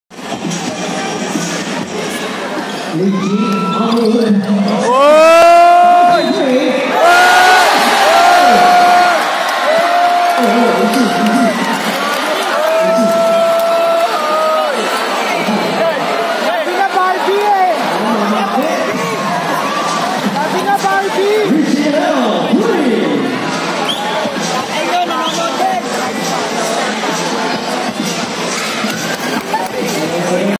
Ricci Rivero Naglaro Sa Laguna
Ricci Rivero Plays in an exhibition game in Sta Rosa Laguna .

Keep Safe and God bless You all.